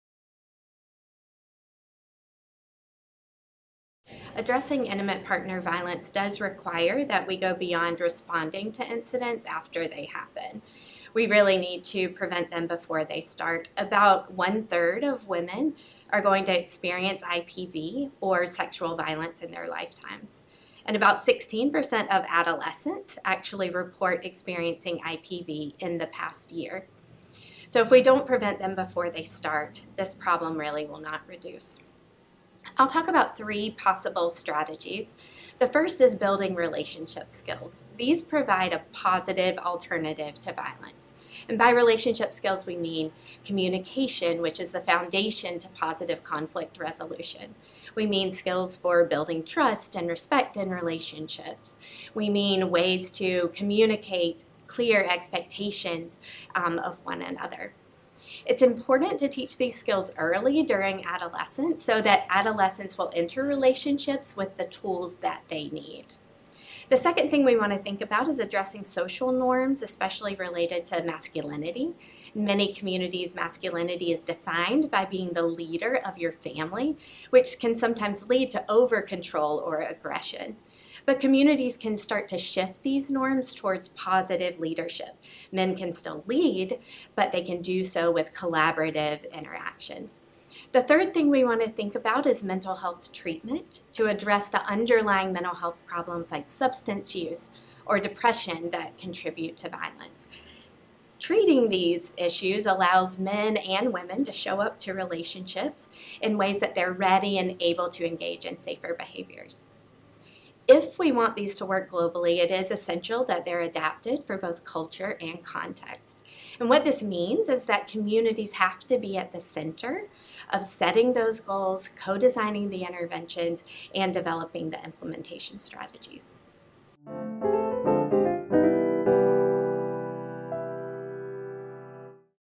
Three Strategies for Preventing Intimate Partner Violence
The death of Ugandan Olympic runner Rebecca Cheptegei, who was doused in kerosene and set on fire by her former partner, in September was a reminder of an all-too-common problem: intimate partner violence, that nearly one third of women ages 15 to 49 have experienced across the globe. In this Quick Take, Duke Center for Global Mental Health Director Eve Puffer shares three strategies for preventing IPV that helps men unlearn harmful behaviors and teaches adolescents positive skills that can carry into dating and relationships later in life.